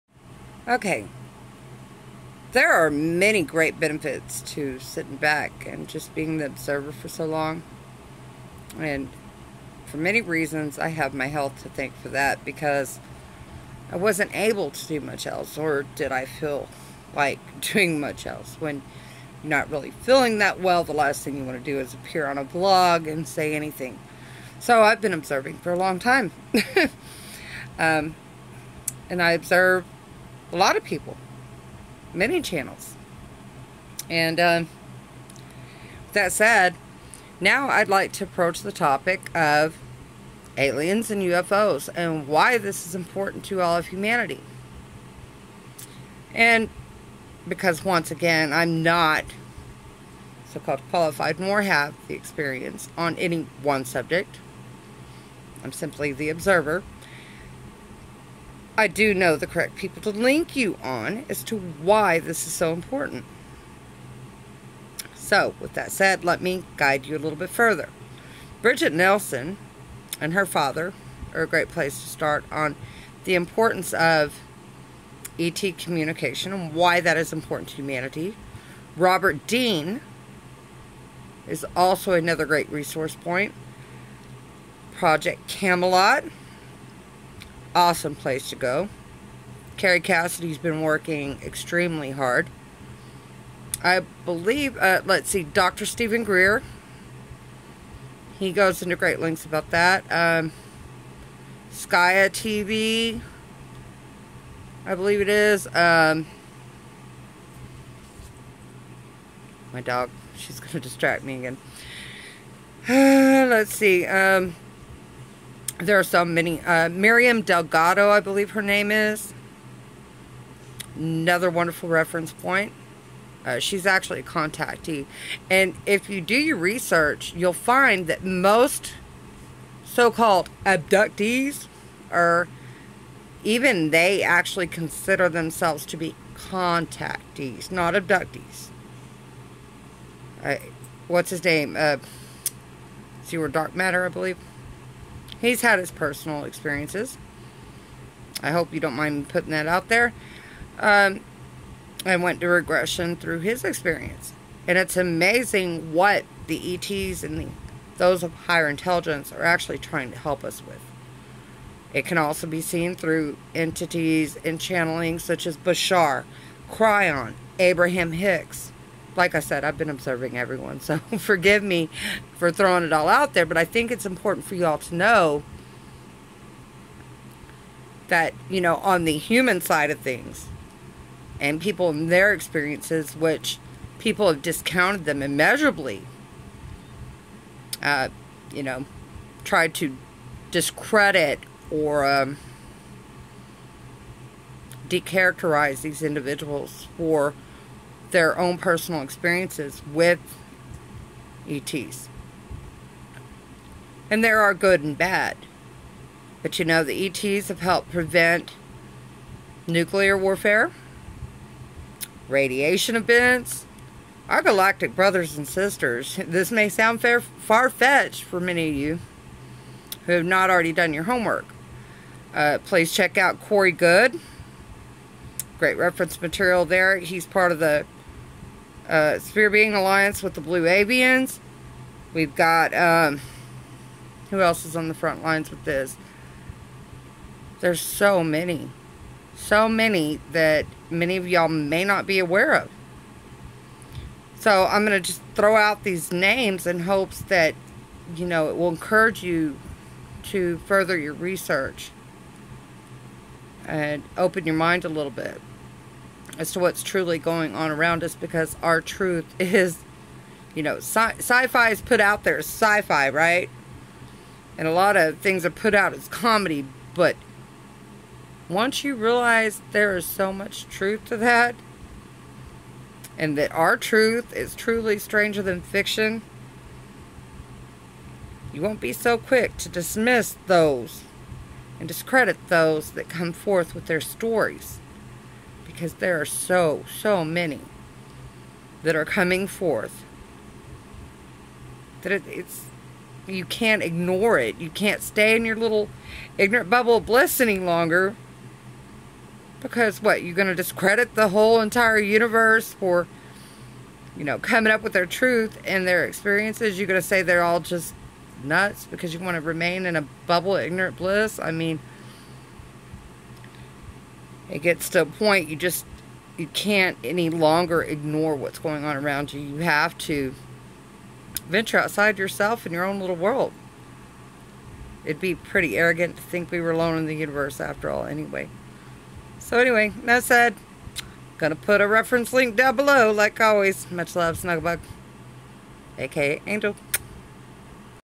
Why is E.T. contact so relevant?
I've named numerous individuals whom all of you may research as to how vital E.T. contact is so prevalent to humanity. I will also provide a link or two in the comments section.